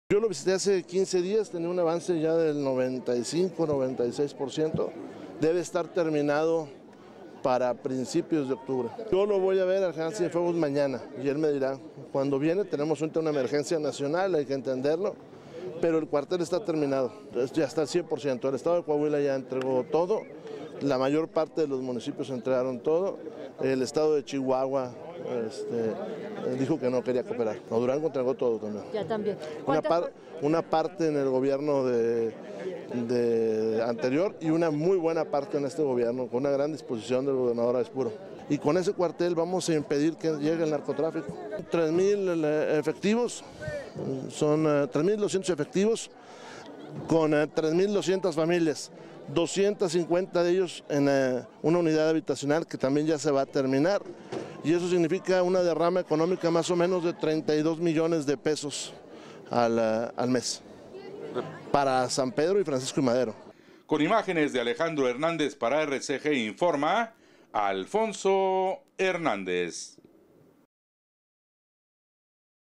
Terminada construcción de Mega Cuartel
Torreón, Coah; Prácticamente quedó concluida la construcción del Mega Cuartel Militar en el municipio de San Pedro, Coahuila, que albergará a más de 3 mil 200  efectivos de la Policía Militar, informó el gobernador Rubén Moreira Valdez.